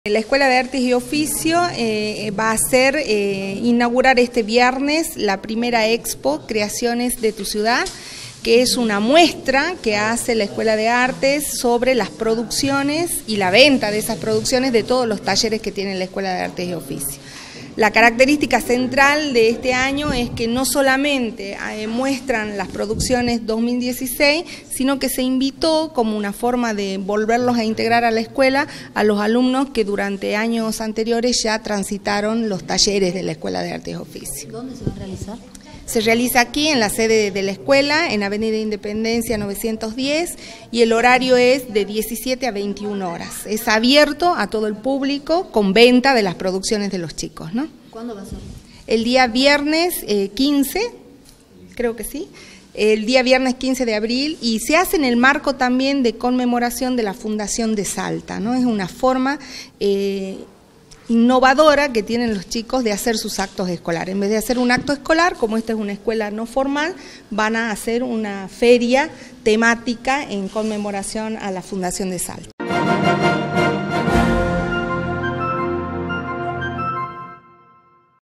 1° EXPO CREACIONES DE MI CIUDAD-TV DOS SALTA
Elizabeth Carrizo, Directora General de Educación de la ciudad de Salta. (DOS EN LA NOTICIA)